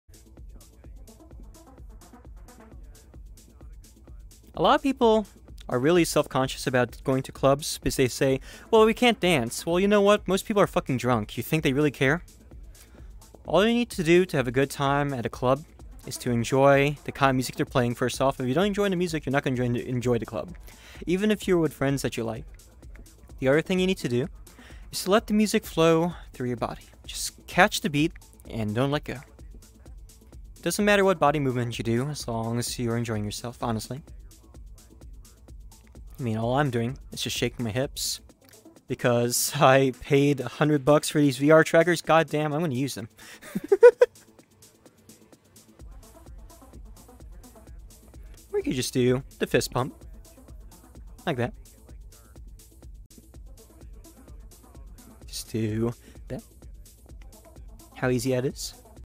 Flat tender vtuber encourages you to curb your self-consciousness.
POV: You're a wallflower in a club and a tipsy flat tender takes you to the rafters to talk.